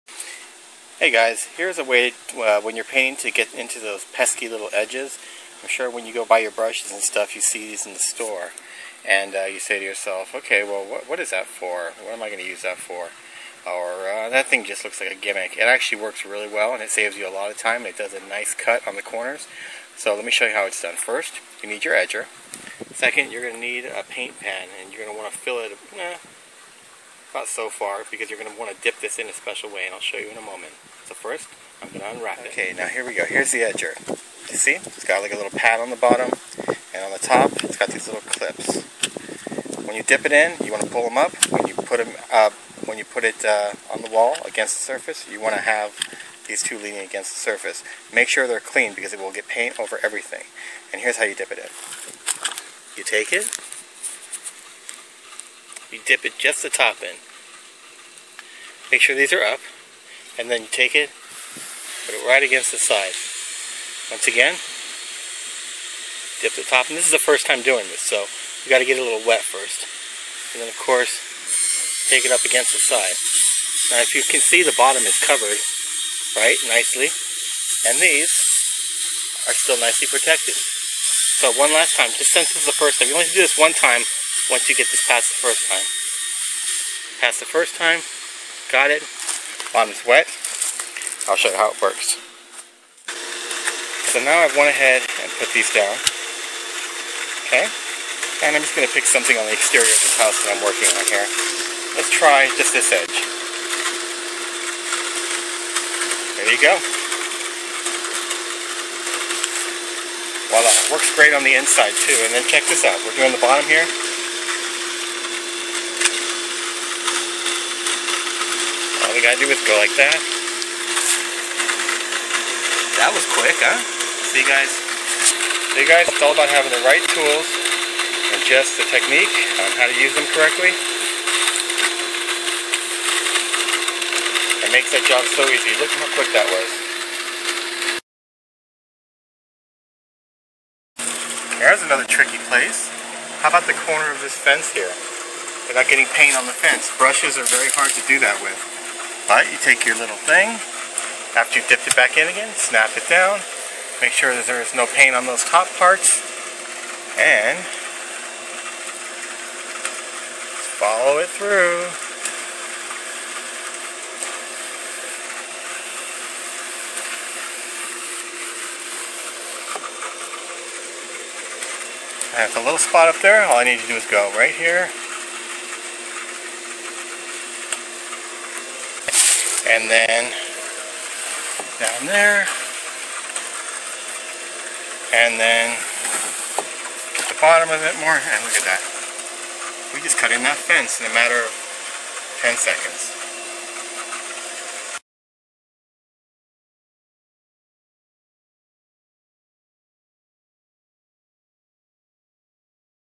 How to use a paint edger - Saves so much time and effort!
Forget using that brush for those stubborn corners. Using an edger will make those edges come out nice and clean.

Samsung WB250F
Lighting Kit